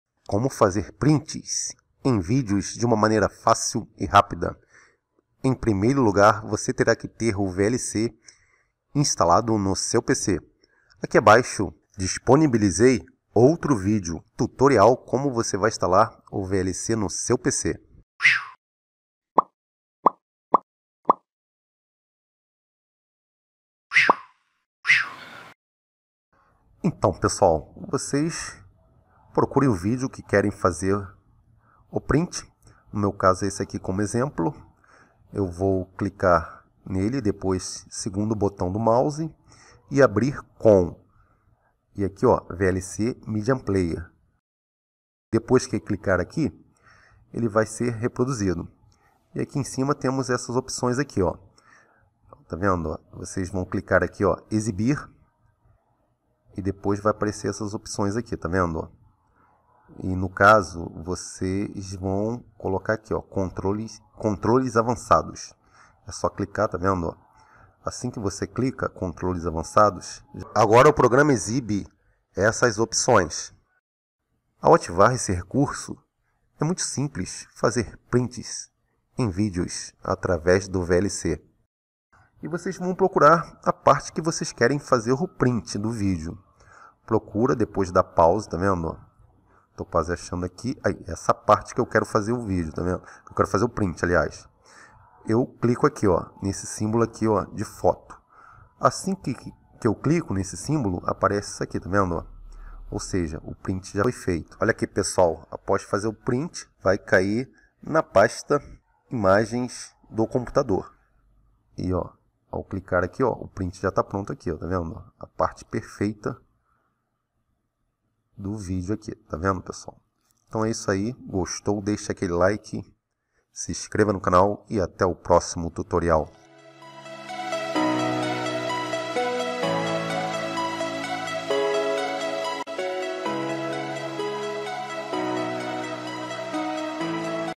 Como Fazer Prints em vídeos - Como tirar prints de vídeos com o VLC no PC
Como Fazer Prints em vídeos -Como Fazer Prints de vídeos -print- print no pc-
▬▬▬▬▬▬▬▬▬▬▬ஜ۩۞۩ஜ▬▬▬▬▬▬▬▬▬▬▬▬▬
▓▓▓▒▒▒░░░ Abaixo Link relacionado ao vídeo  ░░░▒▒▒▓▓▓
▬▬▬▬▬▬▬▬▬▬▬ஜ۩۞۩ஜ▬▬▬▬▬▬▬▬▬▬▬▬▬
▬▬▬▬▬▬▬▬▬▬▬ஜ۩۞۩ஜ▬▬▬▬▬▬▬▬▬▬▬▬▬